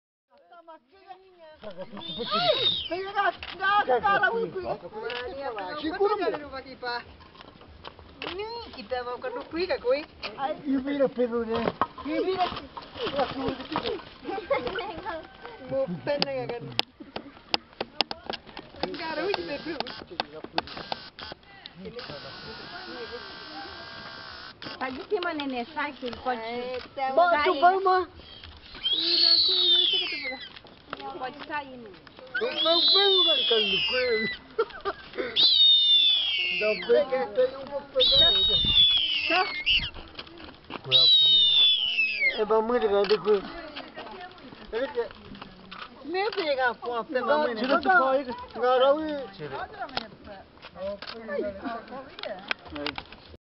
A fuga e a captura do Gavião Real do Pajé Mureyra Asurini - Parte I
O Gavião Real do Pajé Mureyra conseguiu escapar mas logo foi capturado.Tarefa de Asurini que não são fracos não! O Gavião Real é símbolo dos Asurini.  Aldeia Asurini Koatinemo. Rio Xingu - Pará.